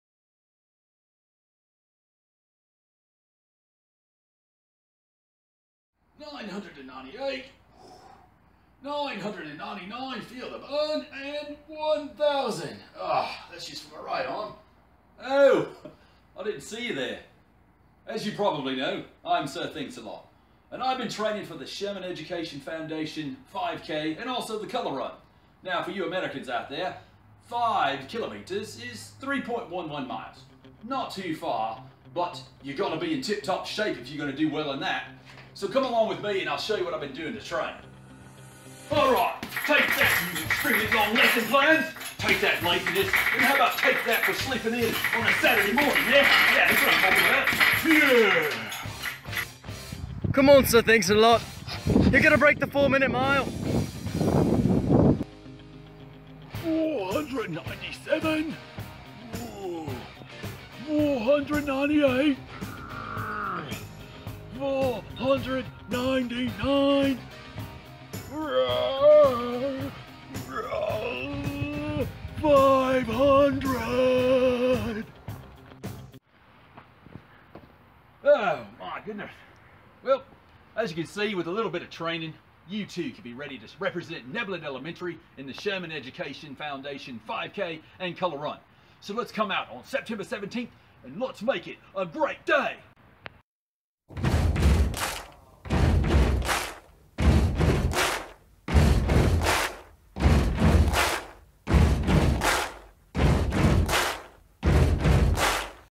SEF Arts Fest Run Sir Thinks a Lot
Sir Thinks-a-Lot is asking for Neblett Elementary's support for the 2016 Sherman Education Foundation Arts Fest Run. Come join your favorite knight on September 17, 2016 at Piner Middle School.